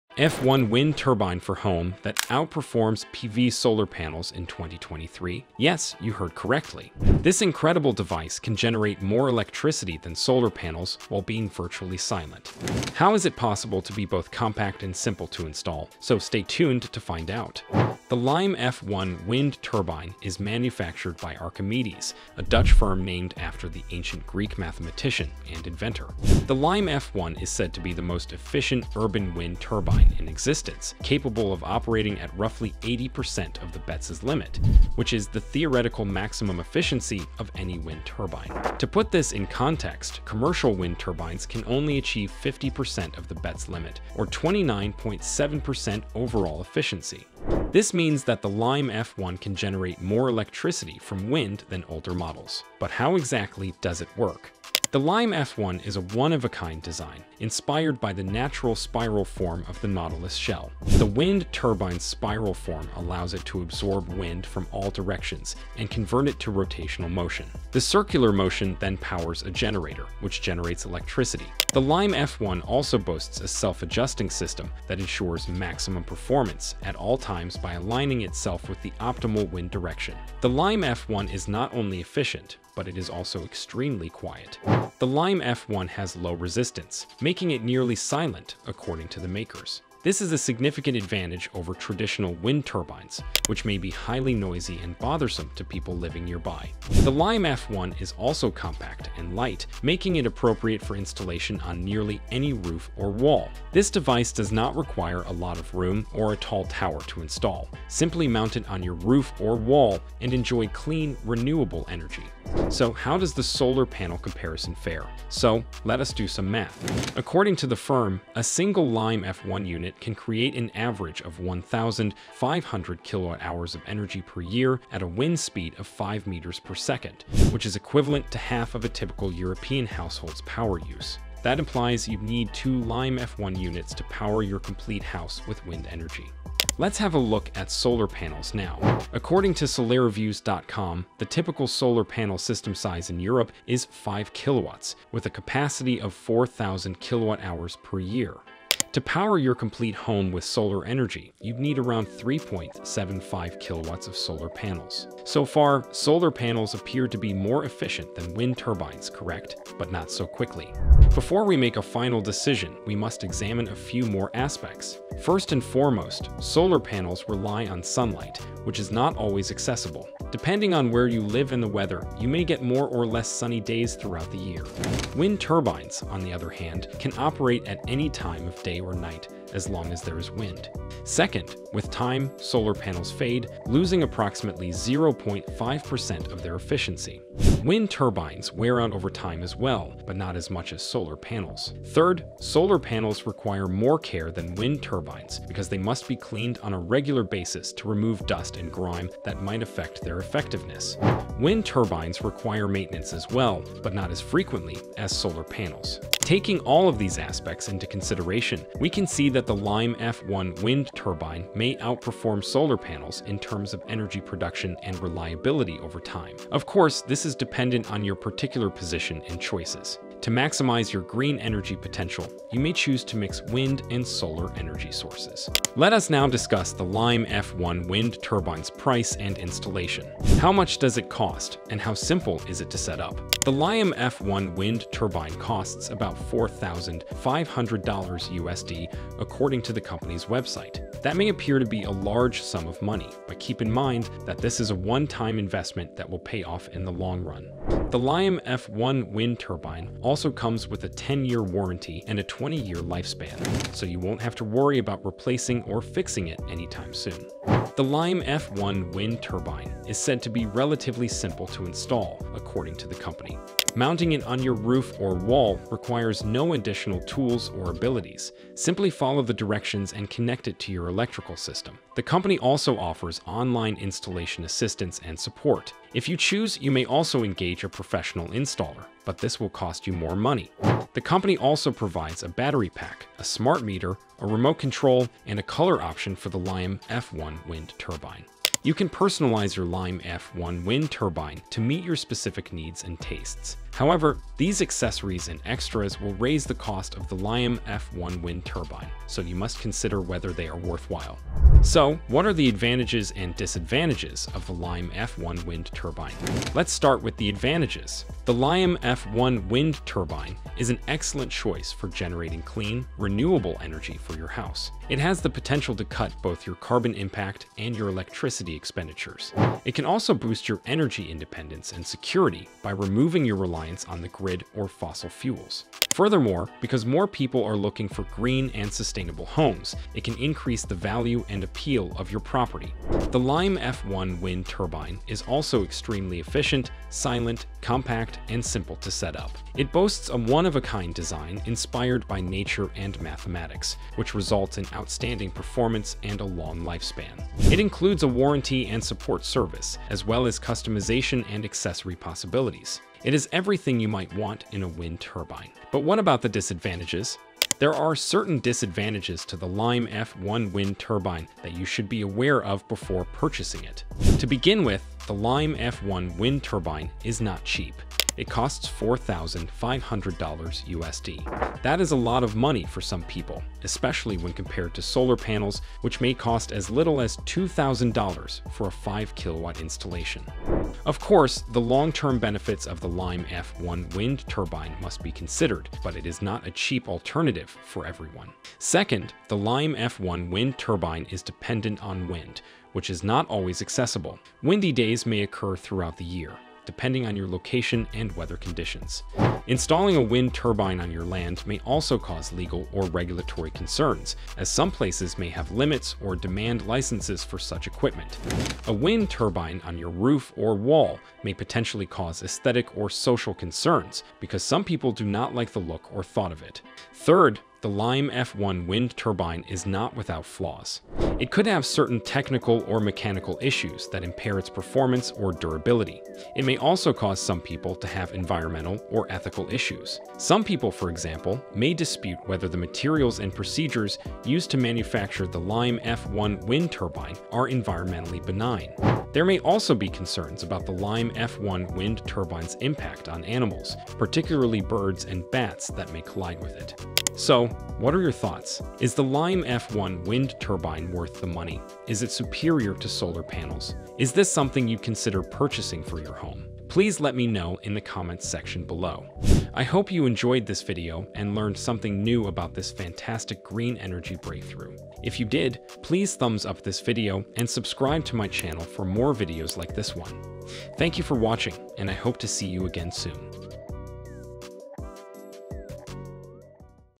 This NEW European Wind Turbine Outperforms All in 2023!! Wind Turbine For Home
In 2023, the new Liam F1 home wind turbine will outperform PV solar panels. Yes, you read that correctly: this incredible technology can create more electricity than solar panels while being nearly silent, tiny, and simple to install. How is this even possible?

Join us on this exciting journey towards a more sustainable future.

Interested in hydrogen and sustainable energy?! Watch this:

NEW Solar Panel That Produces Cheap Green Hydrogen at Home!

🕑Timestamps:
0:00 - Introduction
0:52 - How does the Liam F1 work
1:52 - how does the solar panel comparison fare
3:49 - Cost and Installation
5:00 - Benefits and Drawbacks